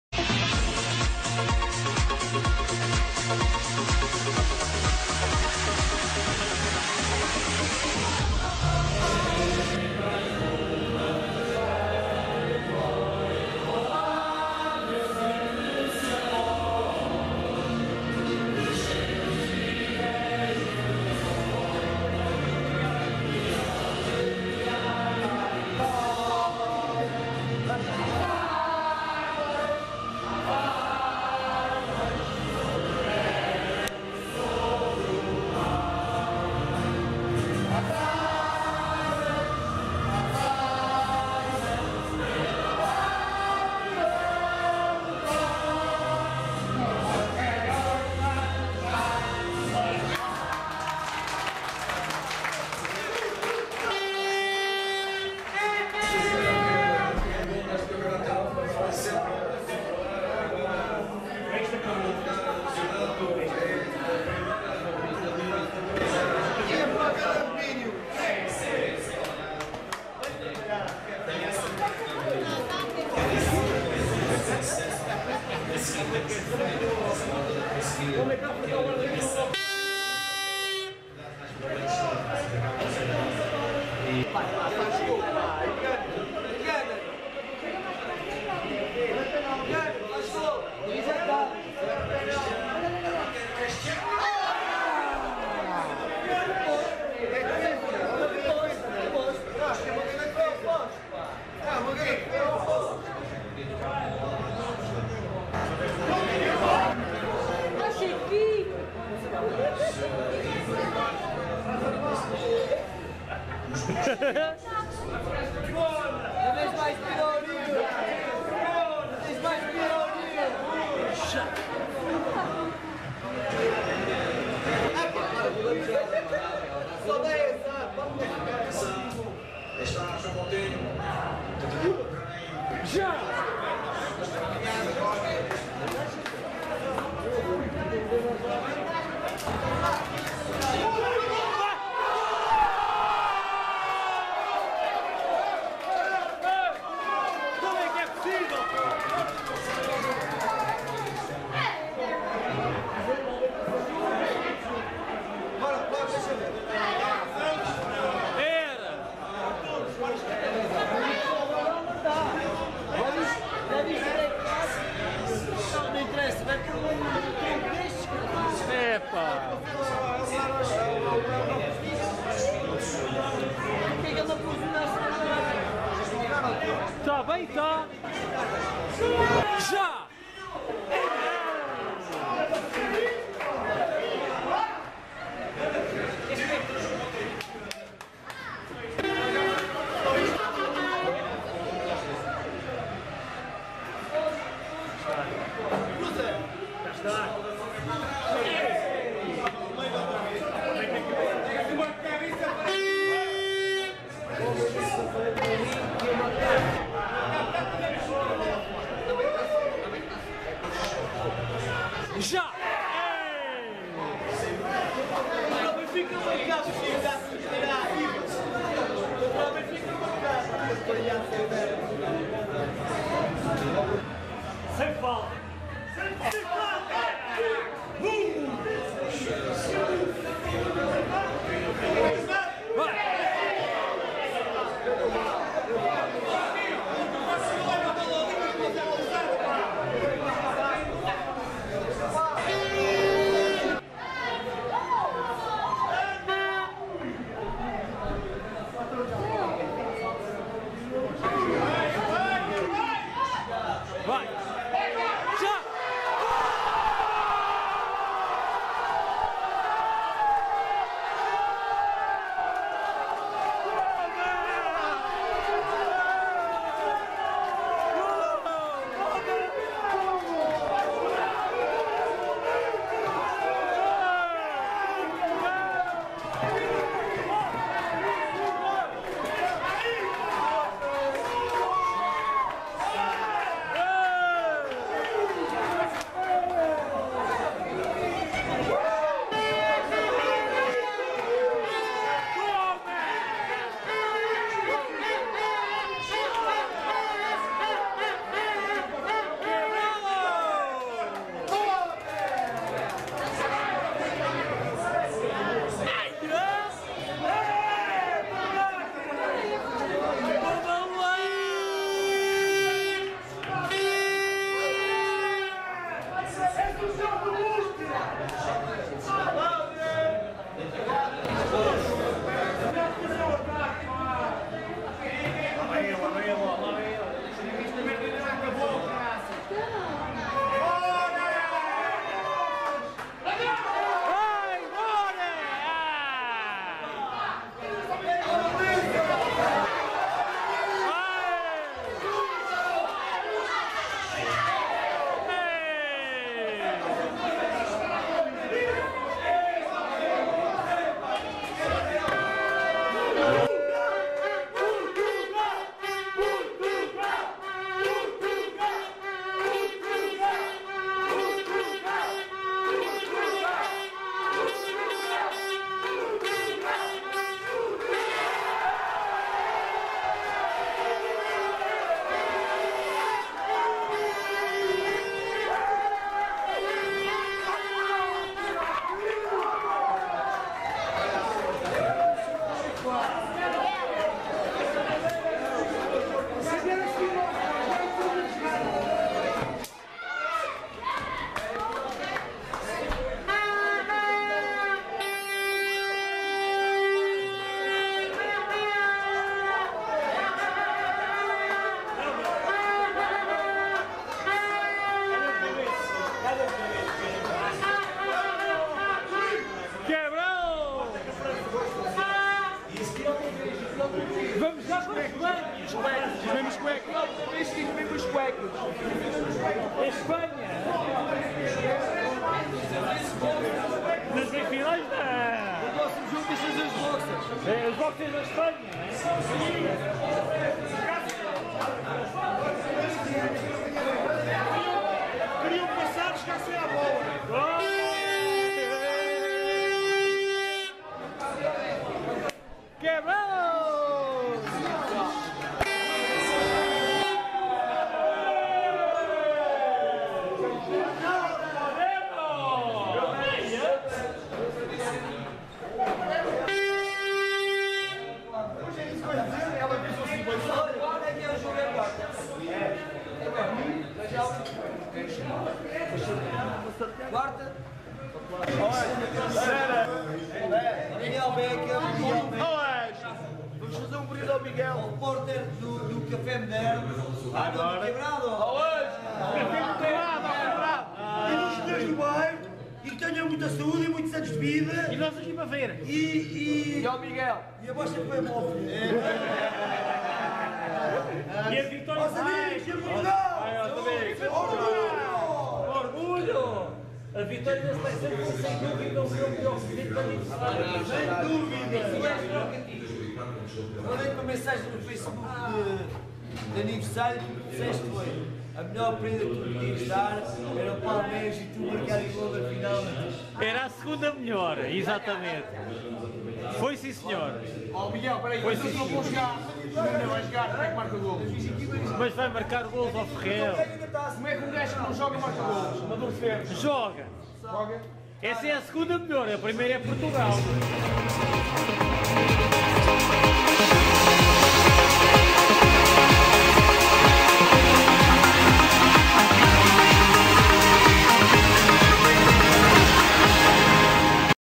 Euro 2012 no Cafe Moderno - Portugal vs Republica Checa
Mais uma vez a Selecção só ficou a ganhar com o Café Moderno devido a este apoio fantastico, concretizando assim o objectivo da passagem ás meias-finais do Euro 2012